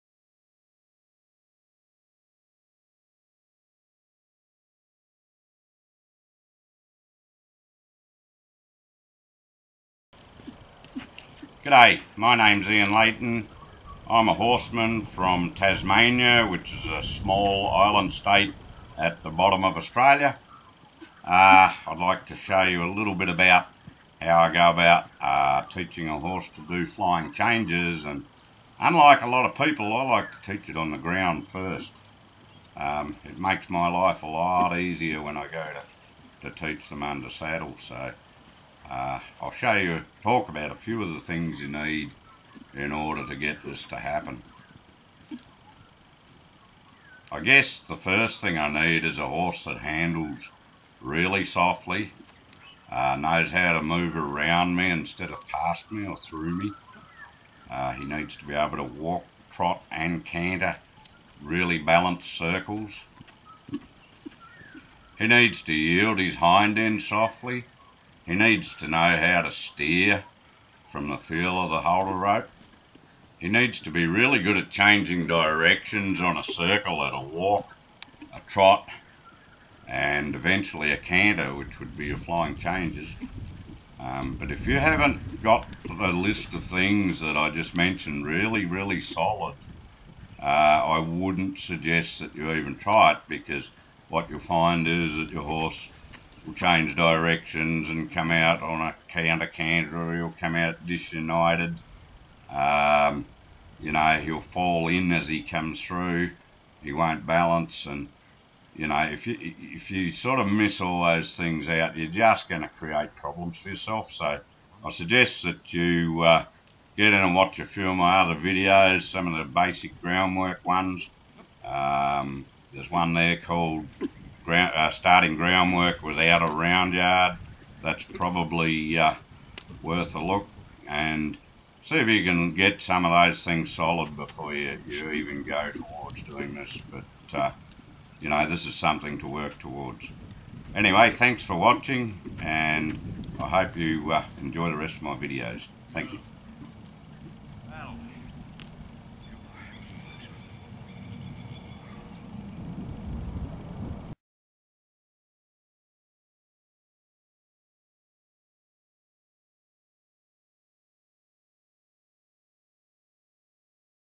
Ian Leighton works on Flying Changes
A short video of Ian Leighton working with a young horse on flying changes.
This is not a tutorial on how to acheive flying changes but through the video Ian explains what you need to teach your horse to get to these.